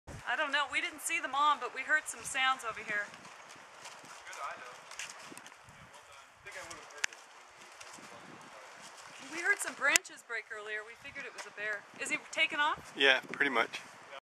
Yosemite National Park Black Bear on the Ostrander Trail
This beautiful blac bear made our hike to Ostrander Lake even more special!  So powerful as it climbs down the tree. We approached it climbing up and as we stared at it from behind the bushes, another one was behind us!  It was awesome!